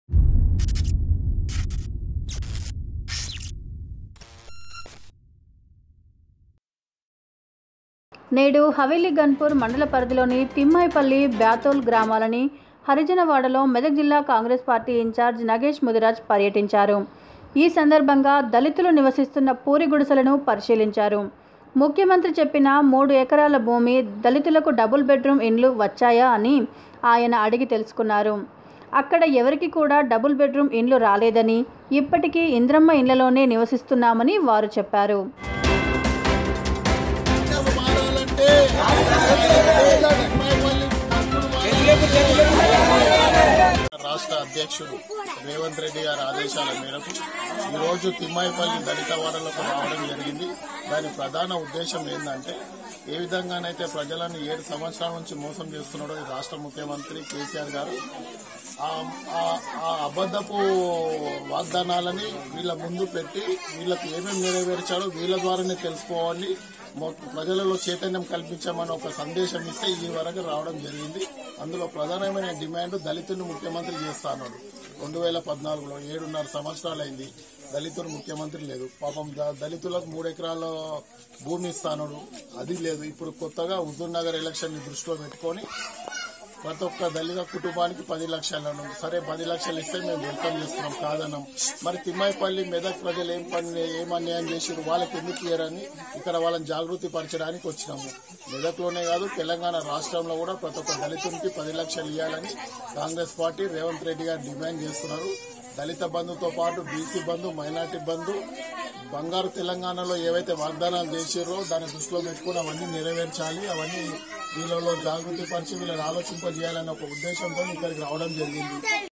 THIRUMALA NEWS 26 08 2021
నేడు హావేలి ఘనపూర్  మండల పరిధిలోని తిమ్మాయిపల్లి, బ్యాతోల్  గ్రామాలని హరిజన  వాడ లో మెదక్ జిల్లా కాంగ్రెస్ పార్టీ ఇంచార్జ్ నగేష్ ముదిరాజ్  పర్యటించారు